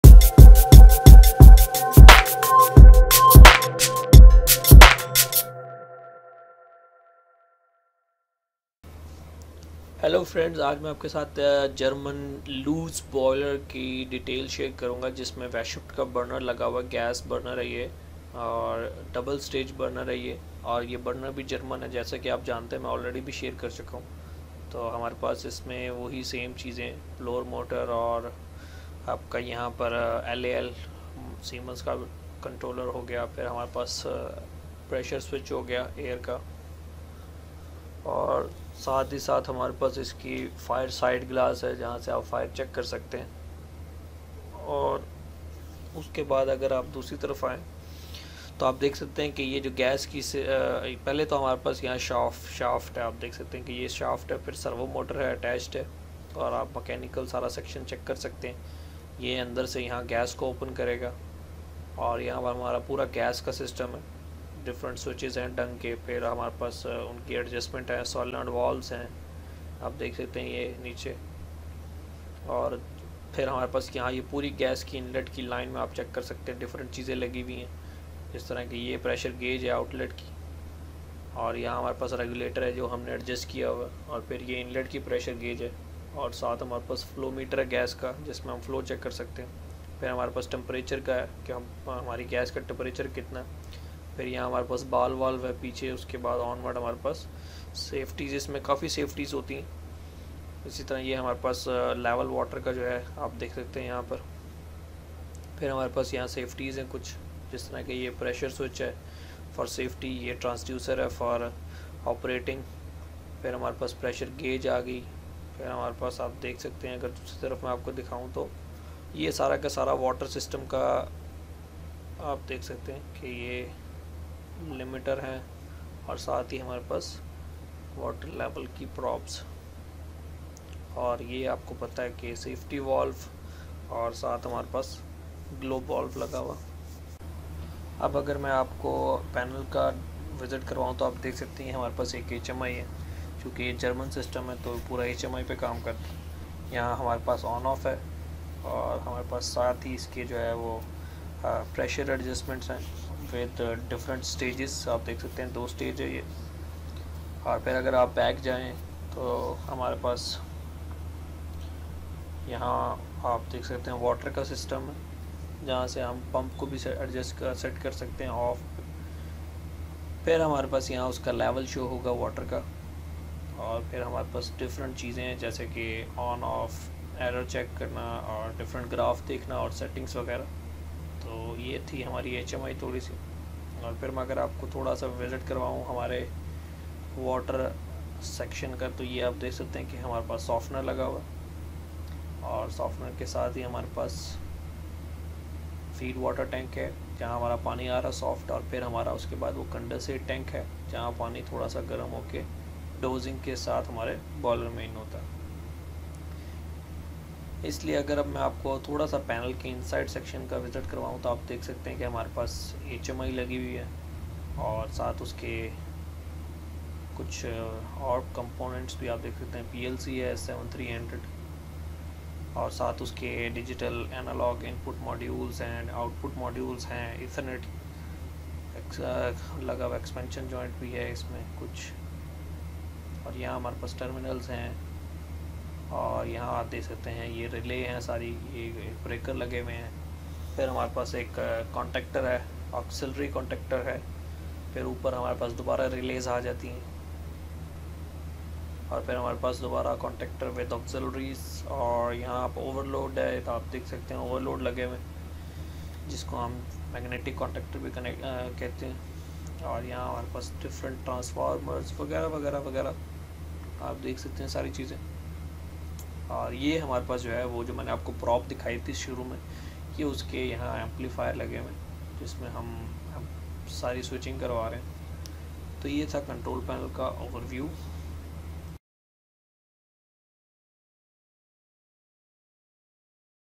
Loos International Boiler and Weishaupt Gas Burner with HMI Control Overview | UzTecho Hindi/Urdu
Welcome to UzTecho Pakistan Youtube Channel.